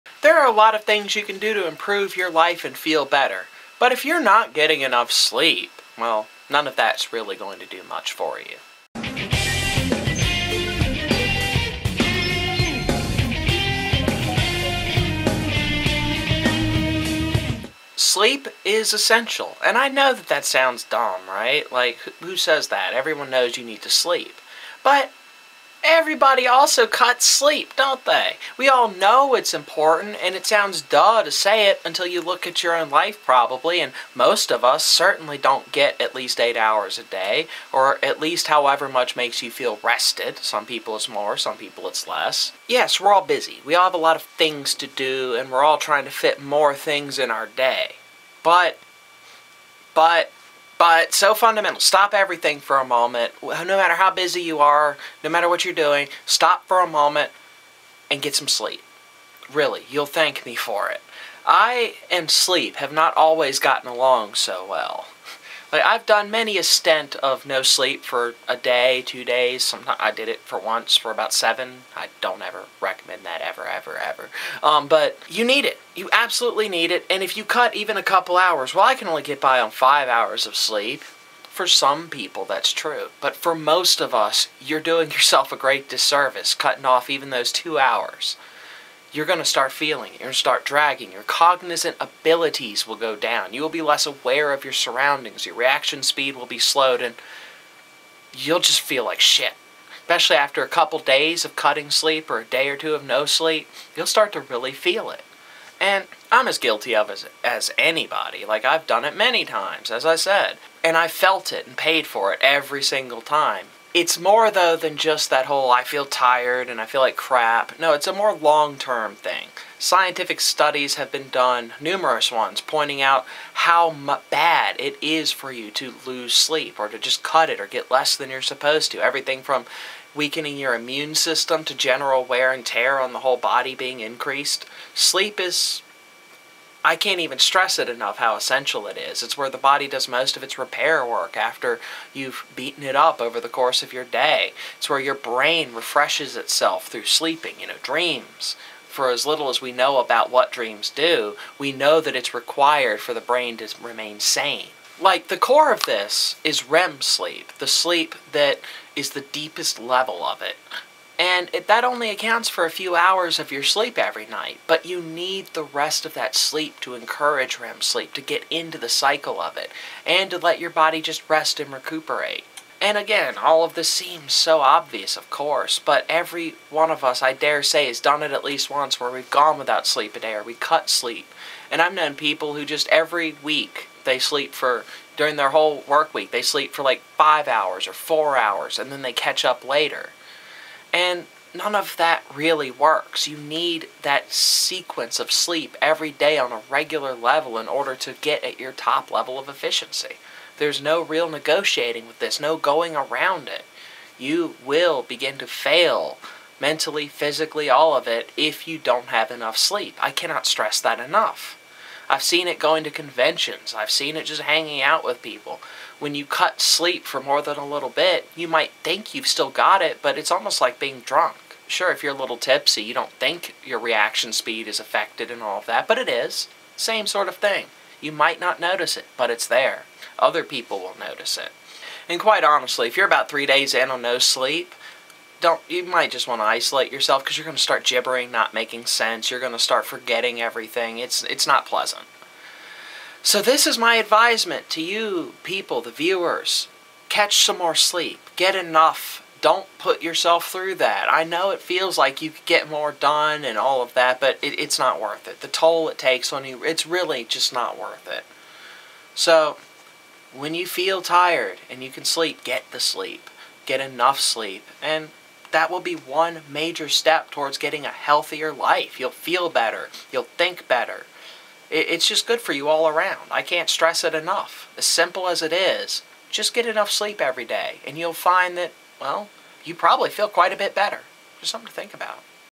Get Some Damn Sleep!
If you aren't getting enough sleep, everything in your life will suffer for it. So make time for it.